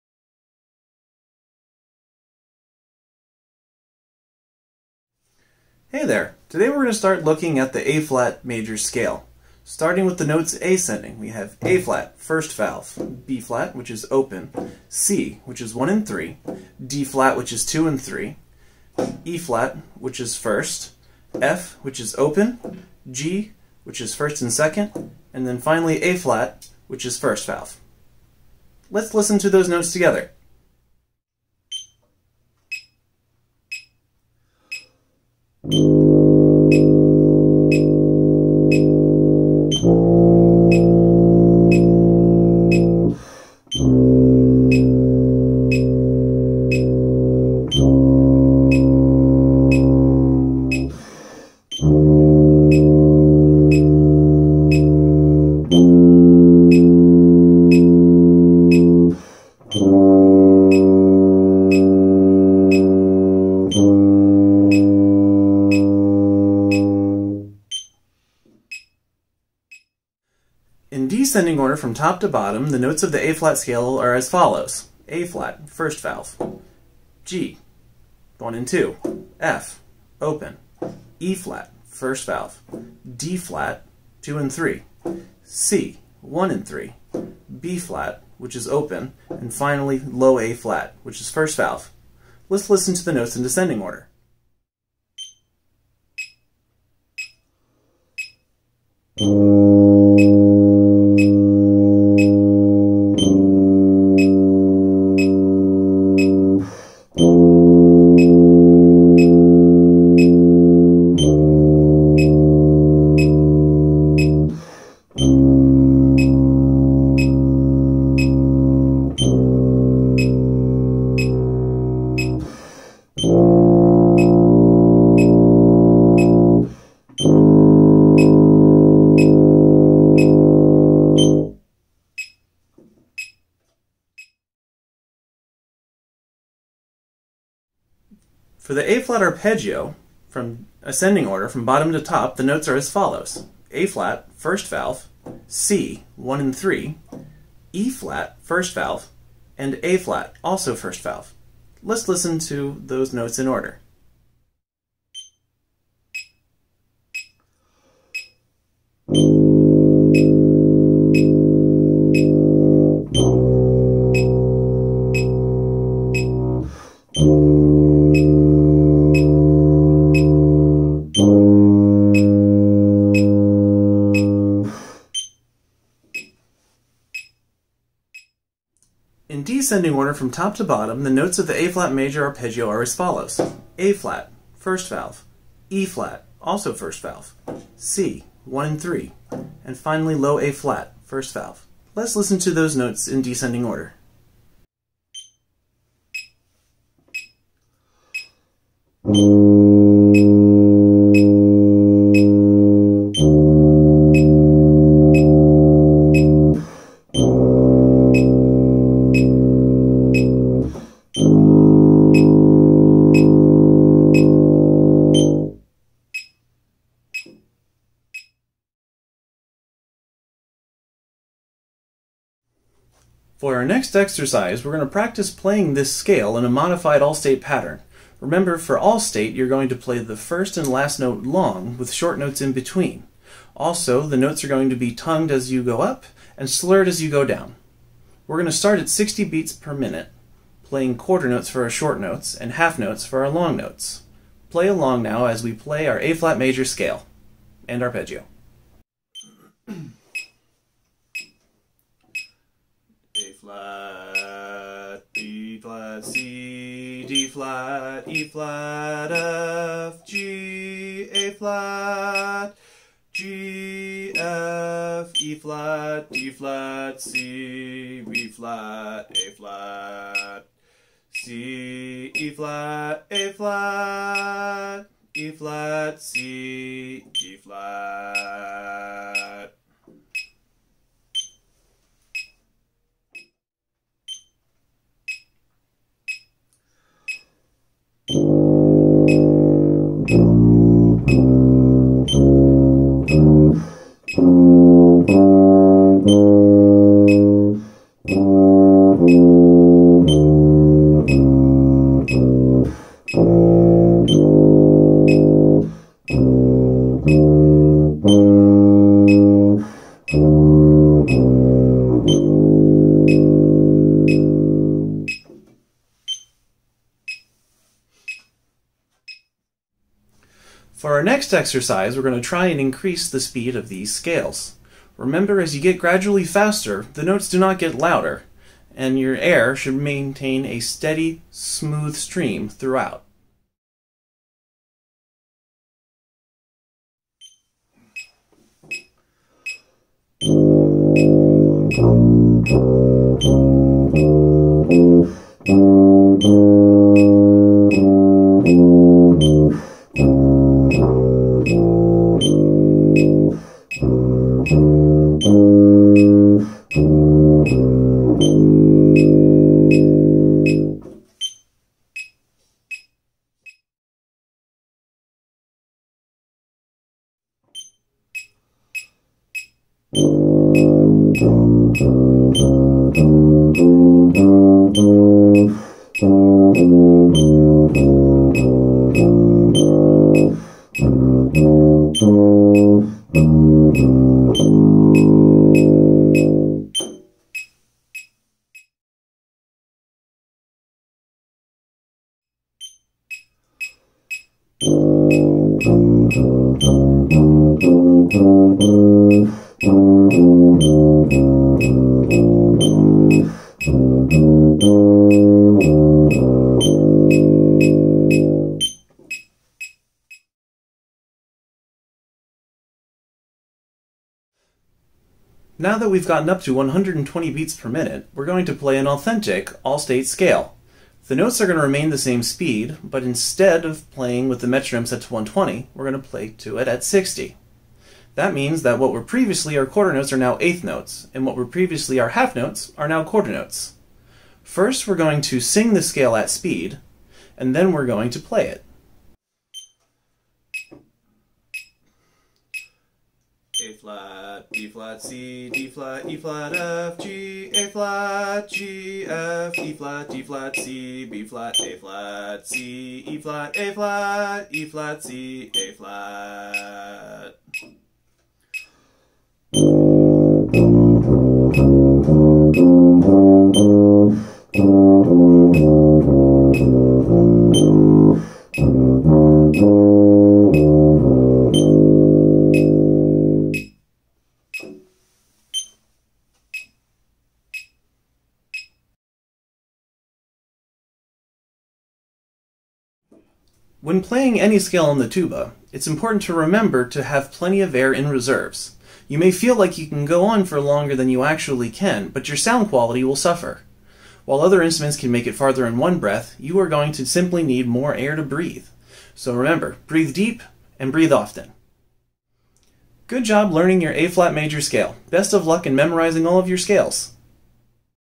Tuba Ab Major Scale
This video is designed to help young tuba players learn the Ab Major Scale. This video is designed to be learned with the "Band Fu" scale fingering charts.

Instructor:
Drew Prichard
Tuba/Euphonium Professor
Hillsborough Community College
Tampa, Fl